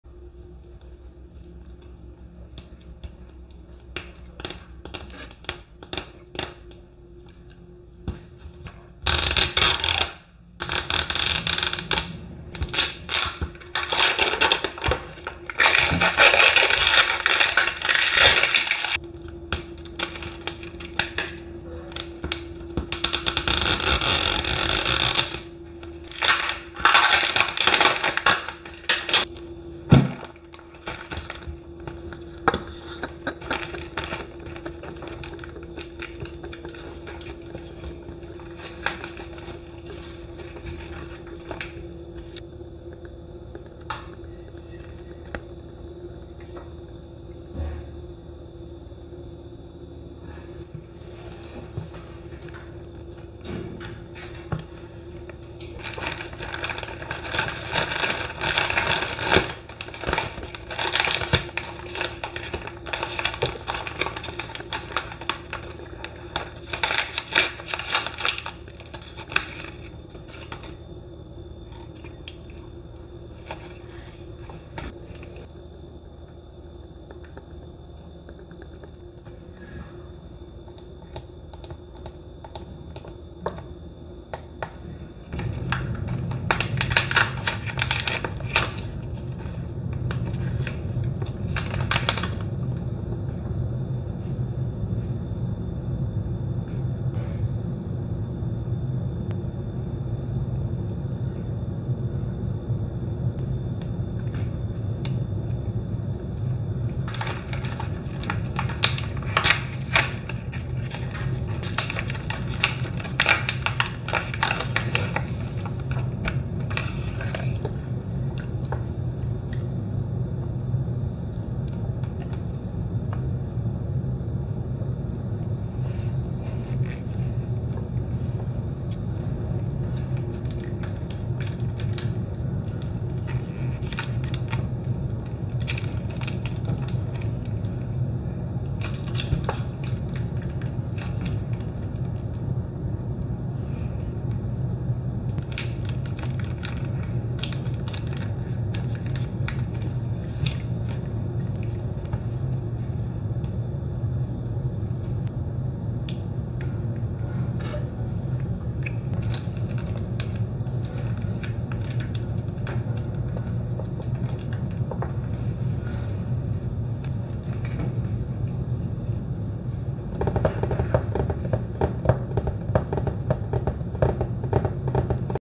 Seriously GROSS Mega Gross Minis Opening ASMR
In this video, we are opening MGA's brand new gross addition to the mini brands line, Mega Gross Minis. 😝 It is like 5 surprise mini foods have all gone BAD! Let's check out which horribly gross items we get!